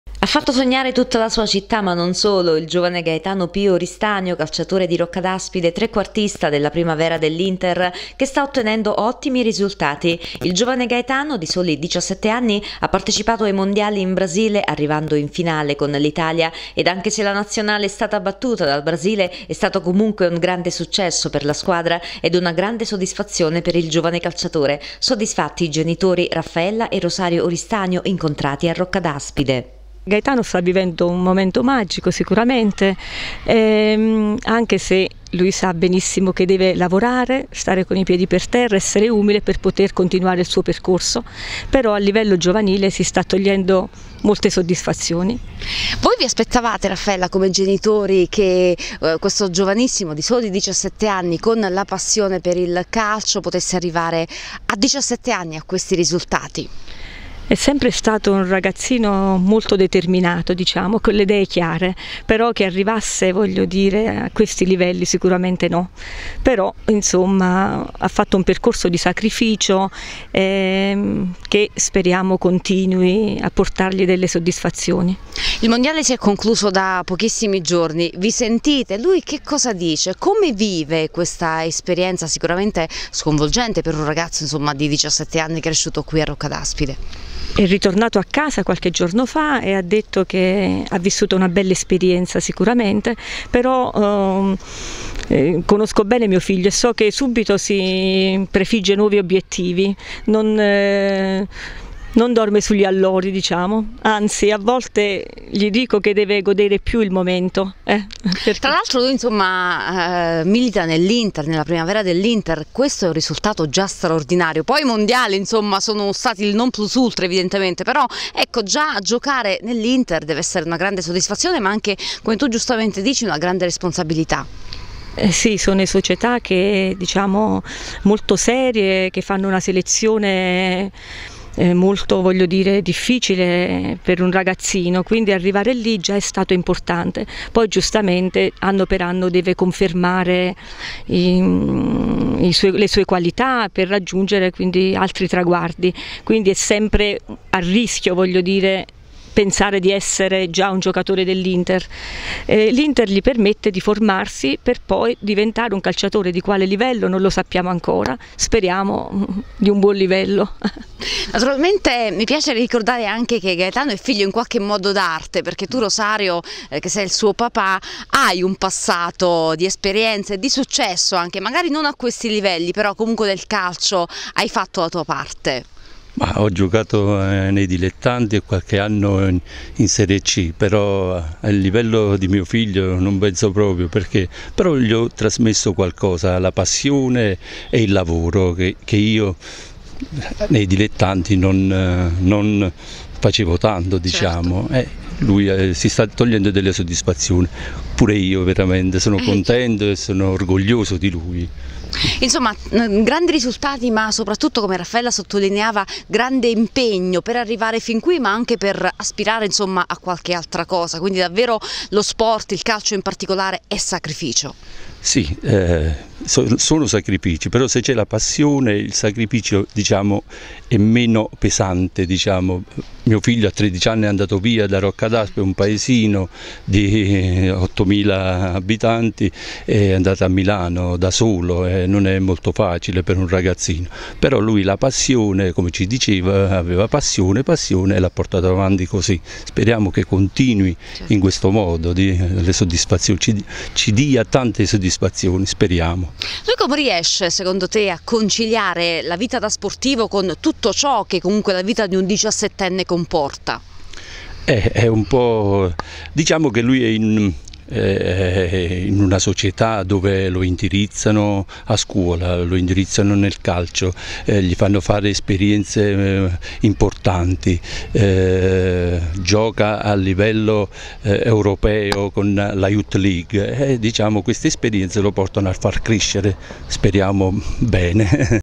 Voci dal Cilento - Roccadaspide, successo e soddisfazioni per Gaetano Pio Oristanio
Roccadaspide, successo e soddisfazioni per Gaetano Pio Oristanio, trequartista della primavera dell’Inter. L’intervista ai genitori

Ha fatto sognare tutta la sua città ma non solo il giovane Gaetano Pio Oristanio, calciatore di Roccadaspide trequartista della primavera dell’Inter che sta ottenendo ottimi risultati. Il giovane Gaetano, di soli 17 anni, ha partecipato ai mondiali in Brasile, arrivando in finale con l’Italia, ed anche se la Nazionale è stata battuta per 2 a 0 dal Brasile, è stato comunque un grande successo per la squadra ed una grande soddisfazione per il giovane calciatore. Soddisfatti i genitori, Raffaella e Rosario Oristanio, incontrati a Roccadaspide.